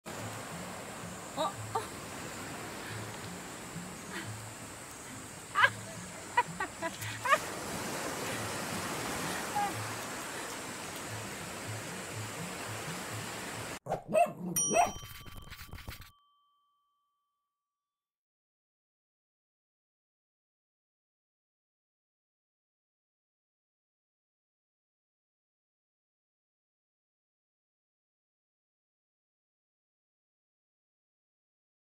Playful pup gets pinched on the nose by angry crab
Watch what happens when a curious and playful pup tries to sniff and play with a crab that has anger issues! Extremely amusing!